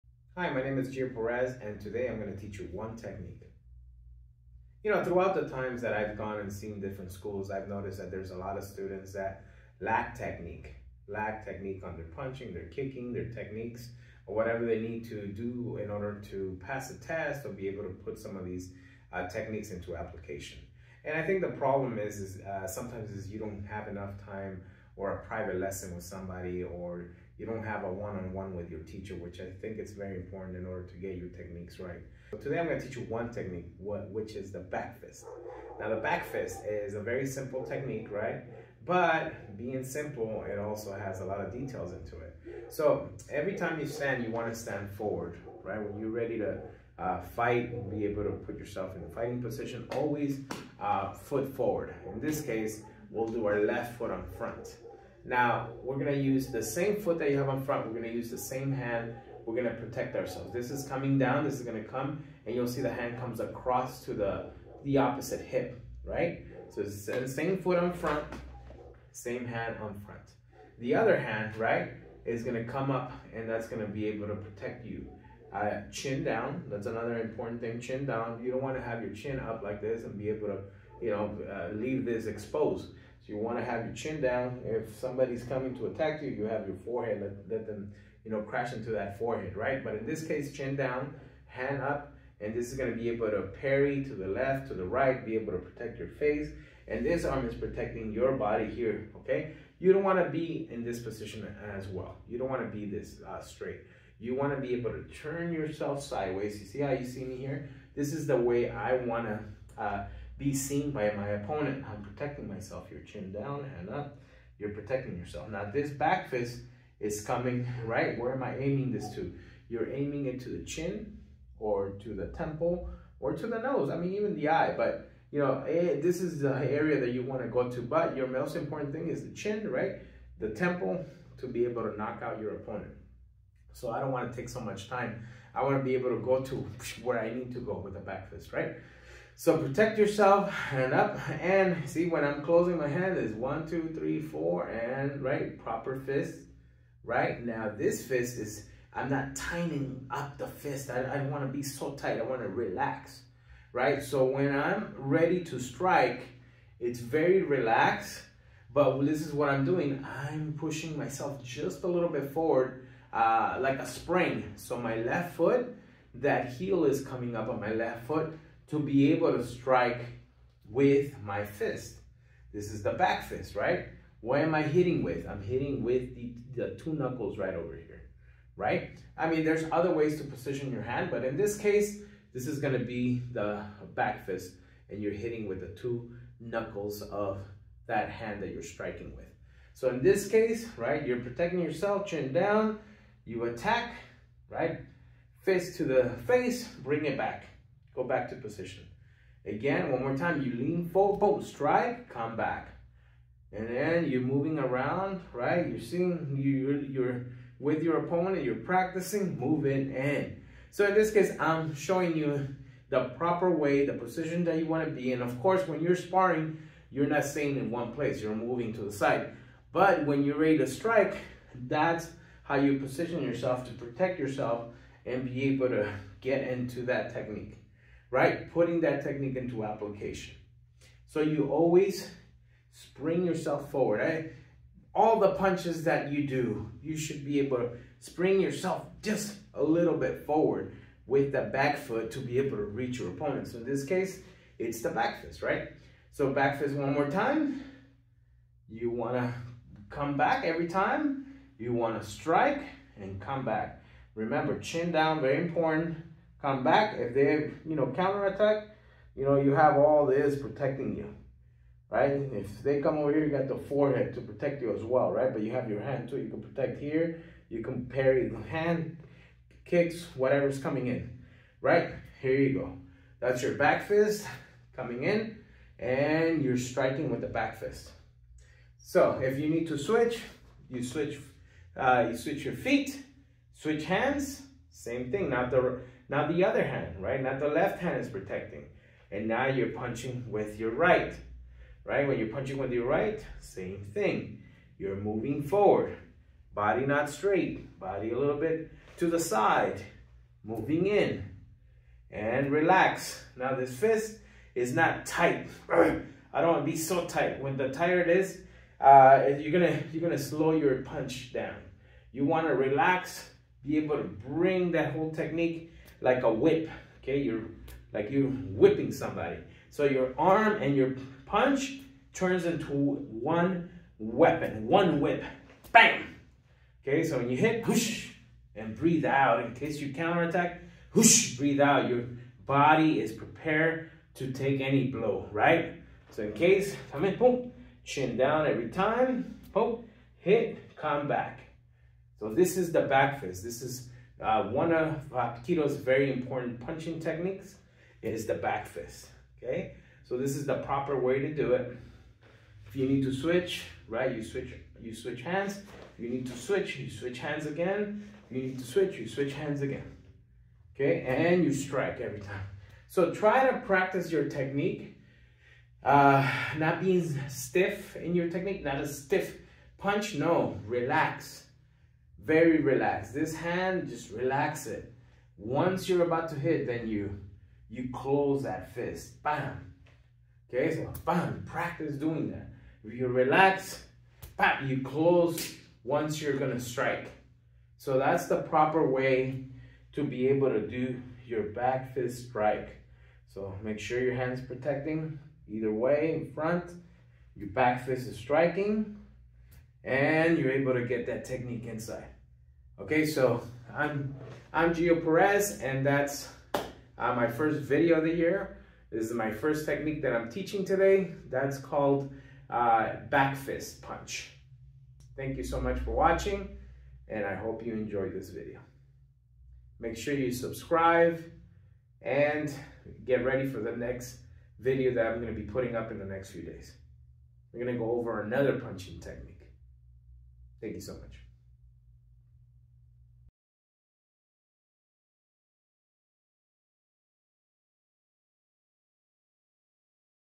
Back Fist Punching technique and application.
Gio Perez Teaching back fist for effective use in application. The back fist is a very simple punching technique, but it has many other details. on this video you see other body movements to defend and attack while using the back fist punch.